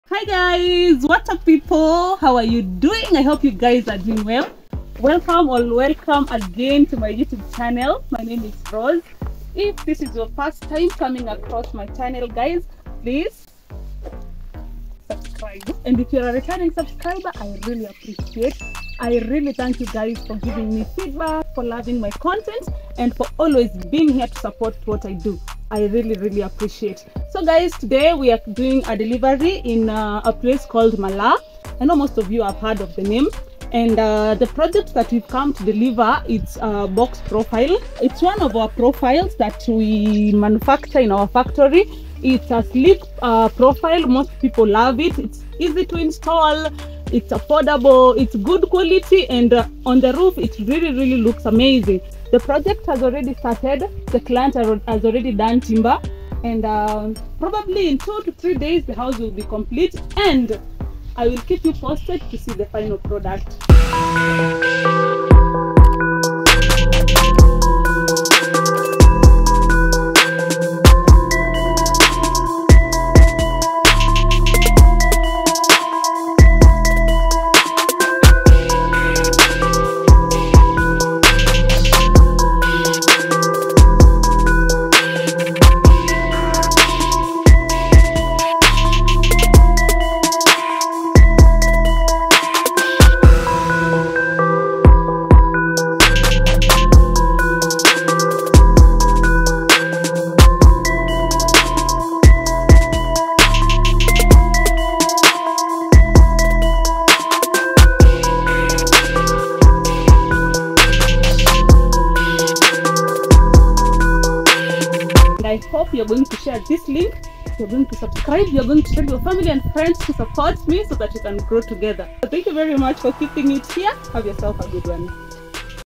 Watch as I Delivers Box Profile Mabati to Malaa
Rose Mueni Delivers Box Profile Mabati to Malaa: Witness the seamless delivery process as Rose Mueni ensures the safe transport of box profile mabati to Malaa.